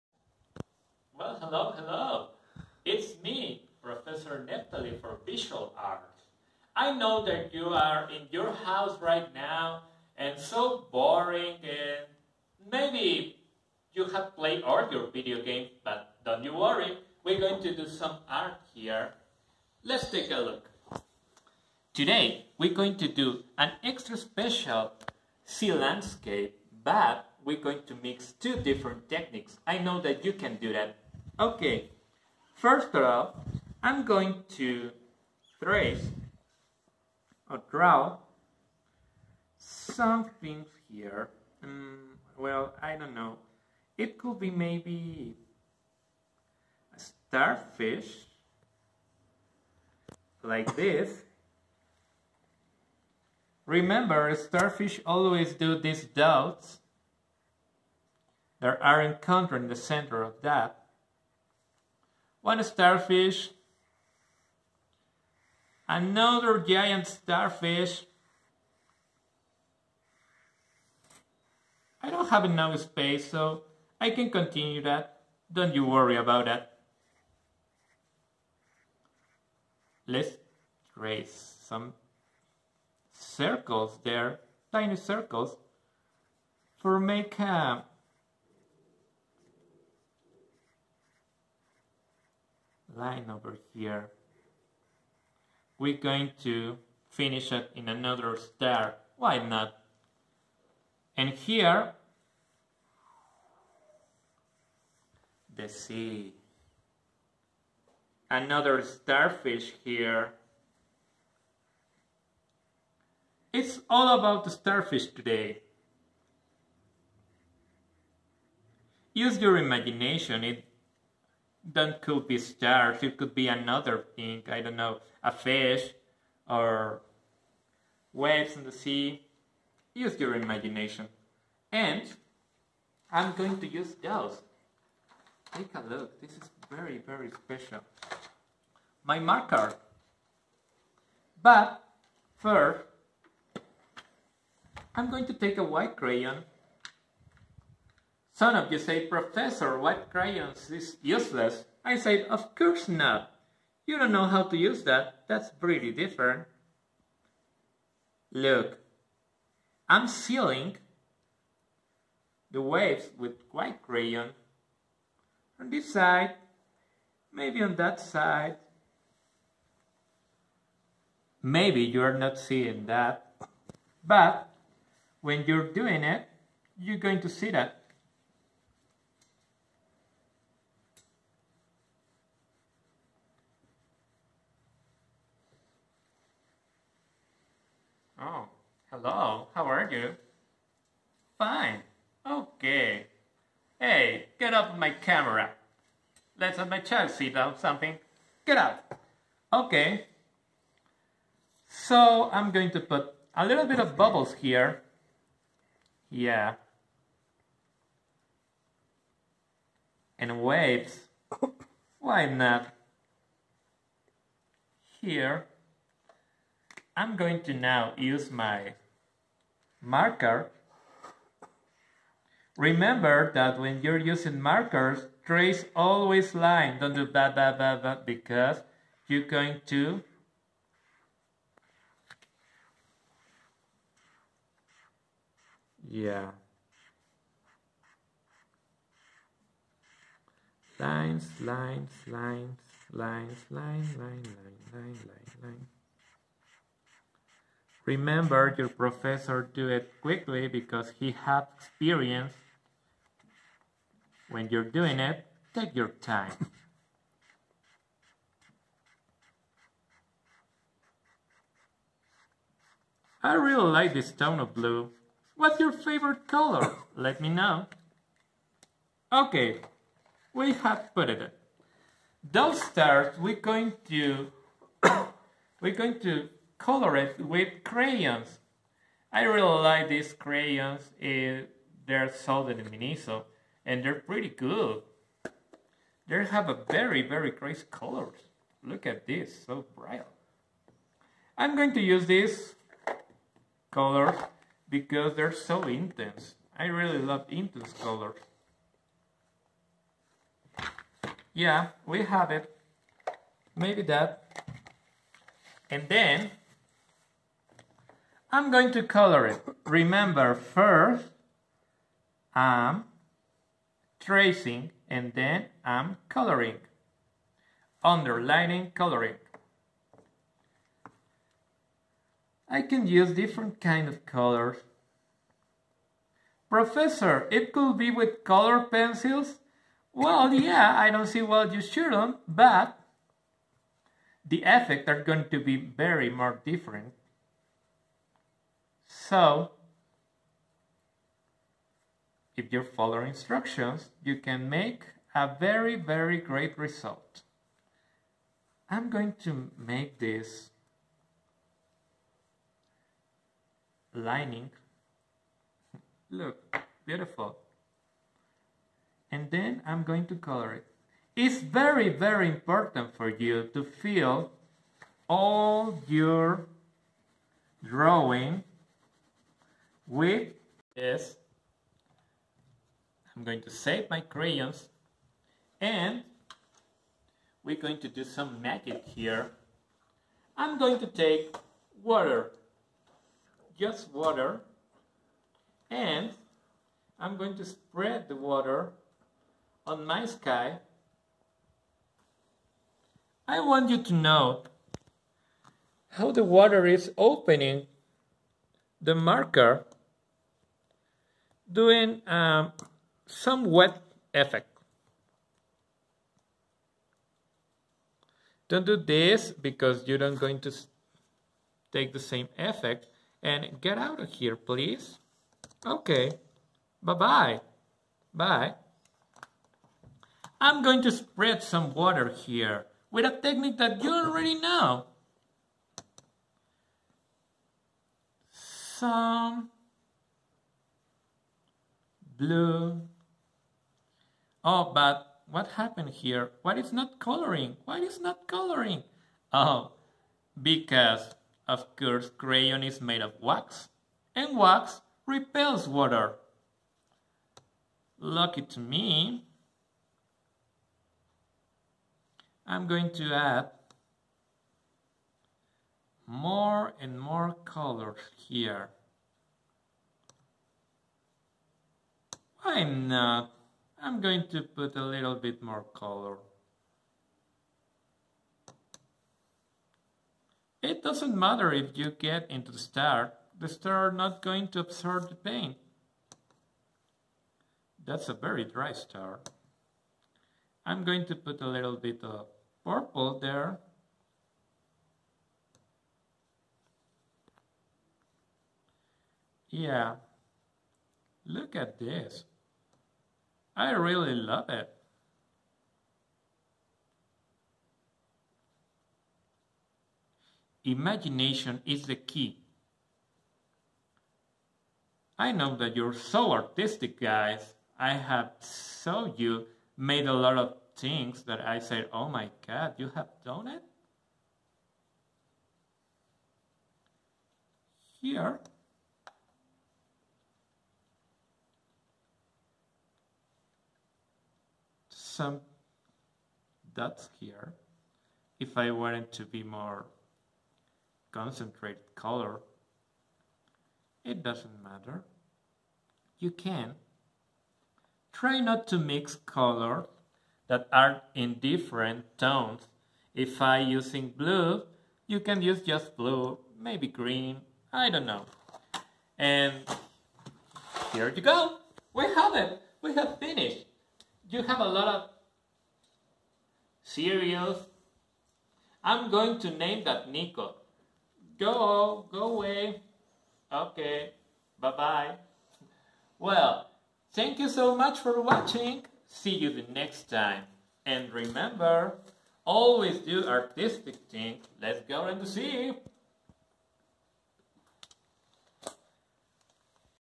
Watercolor Starfish maritime landscape
A simple landscape of sea stars with watercolor, crayons, and water-based markers